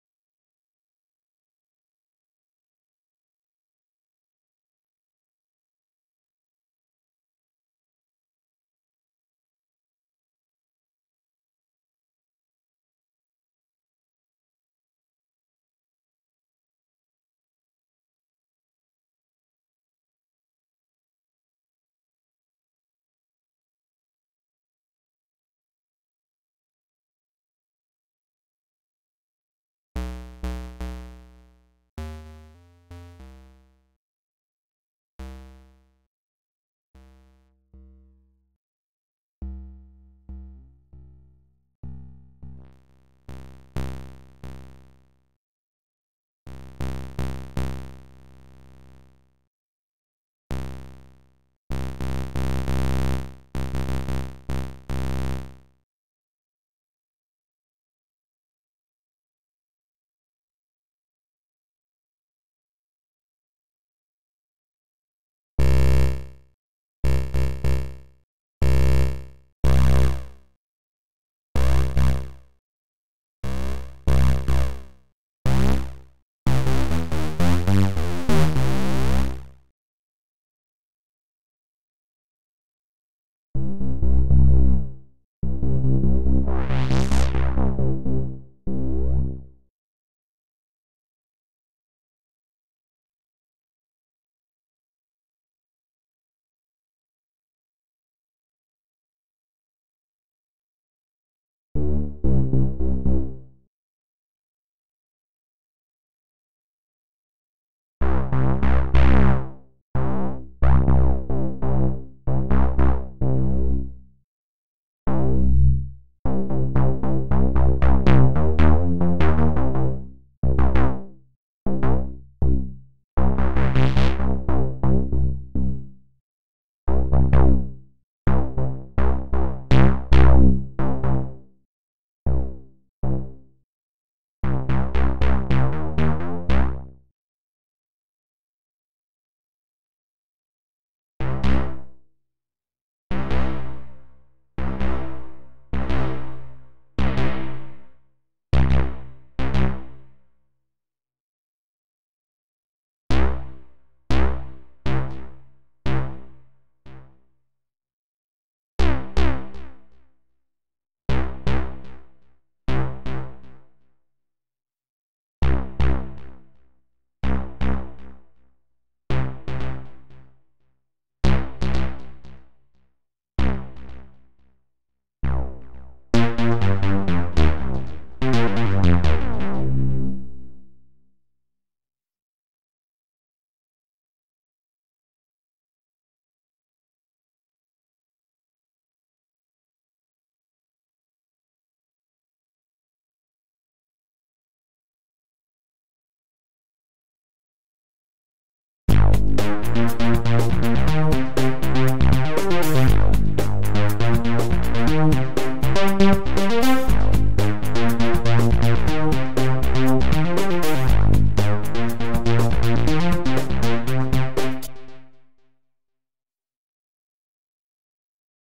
Making a subtractive modular synth from scratch | LKCTools.com
Download REAPER!

ReaRack is a collection of JSFX modules that can be used for modular synthesis exclusively inside Reaper, for free.

ReaPack is a package manager for REAPER.
It eases the download/installation and update of REAPER resources (ReaScripts, JS effects, themes, native extensions, language packs and web browser interfaces).

On the left you can see cubase-like track inspector, that's Heda's Track Inspector which can also be downloaded for free.